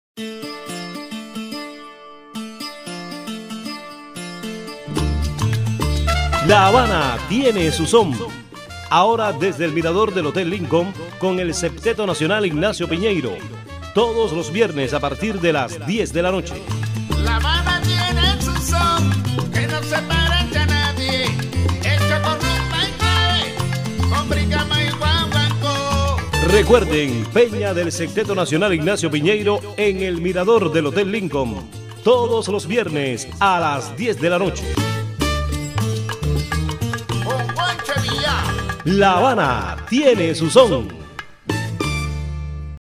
Peña del Septeto Nacional Ignacio Piñeiro - Hotel Lincoln, Habana-Cuba
Todos los viernes a partir de las 22h la Peña del Septeto Nacional Ignacio Piñeiro en el Mirador del Hotel Lincoln, Virtudes No.164 | Esq. Galiano, Ciudad de La Habana, Cuba.